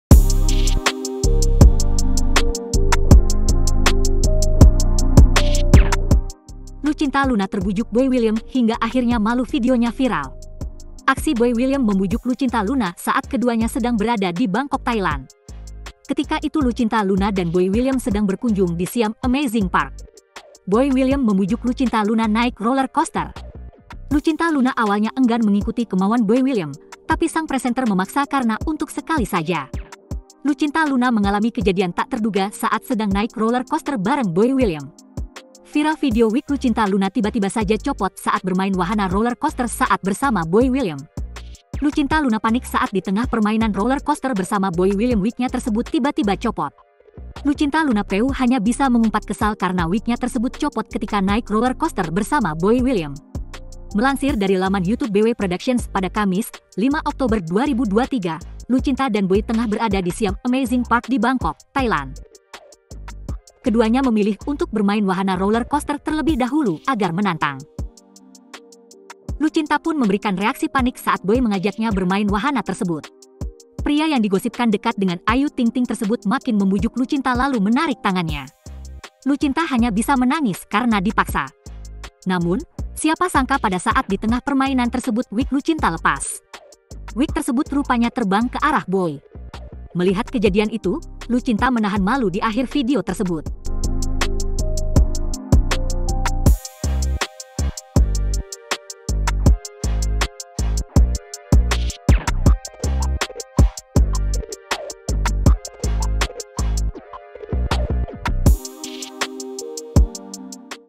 Lucinta Luna Terbujuk Boy William hingga Akhirnya Malu, Videonya Viral
Aksi Boy William membujuk Lucinta Luna saat keduanya sedang berada di Bangkok Thailand.

Ketika itu Lucinta Luna dan Boy William sedang berkunjung di Siam Amazing Park.